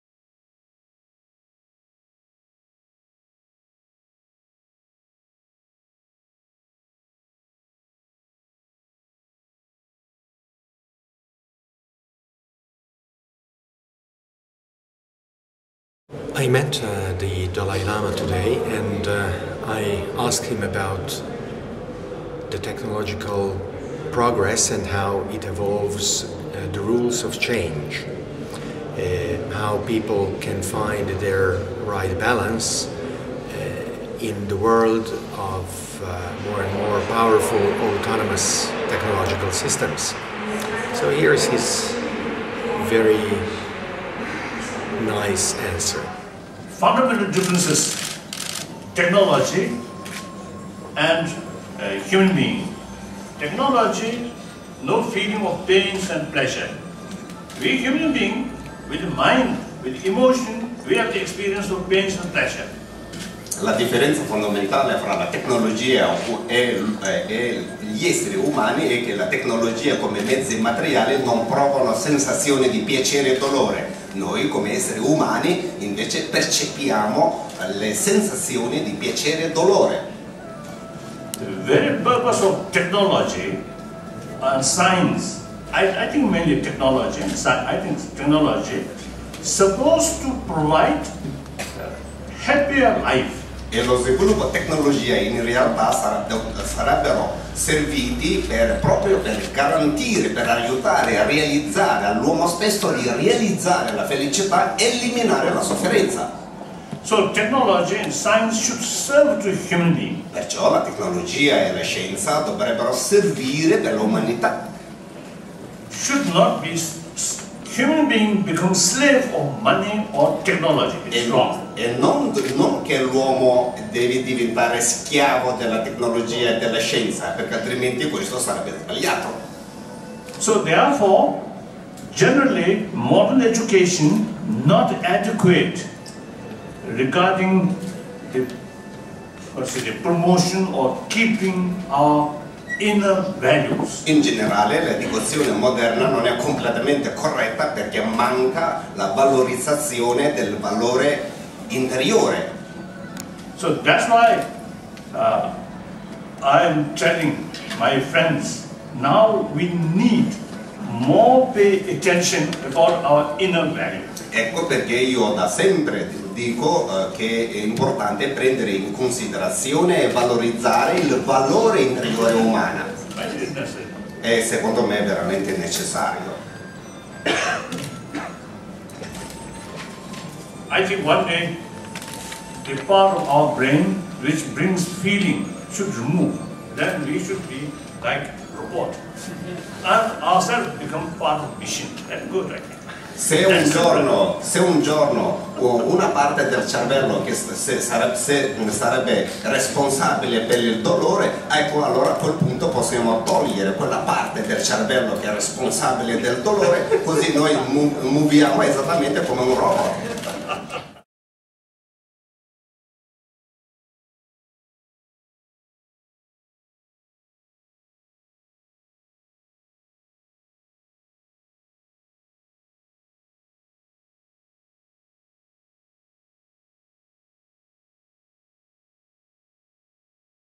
The Dalai Lama on Technological Change
Is the Dalai Lama a transhumanist singularitarian? Watch this video to find out yourself!

Tenzin Gyatso, 14th Dalai Lama on 16 december 2007 in Turin, Italy answering a question by David Orban:

"How can people find the right balance if their adaptability is stretched to its limits by technological progress evolving the rules of change?"